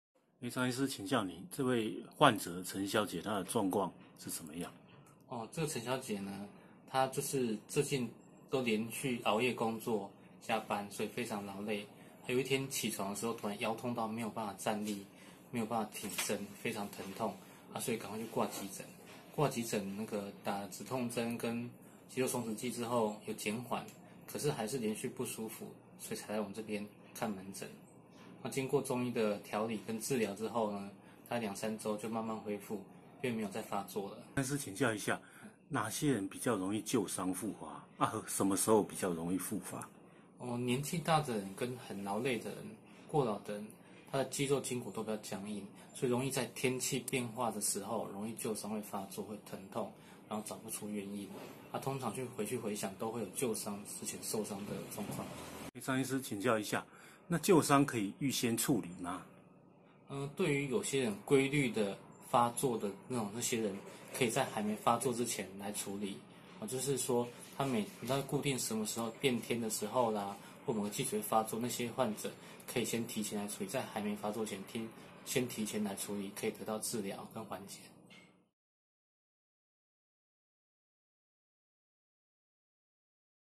醫病》年輕OL連續熬夜 熬成風溼送急診
25歲的陳小姐連日加班到深夜，某日起床突然腰痛劇烈，根本無法站立或坐，只好趕快掛急診，經緊急施打止痛針和肌肉鬆弛劑後疼痛才稍緩，後經醫師診療發現，陳小姐經常熬夜，加上先前車禍舊傷未癒引發風溼，經按摩推拿與用藥3週後才不再疼痛。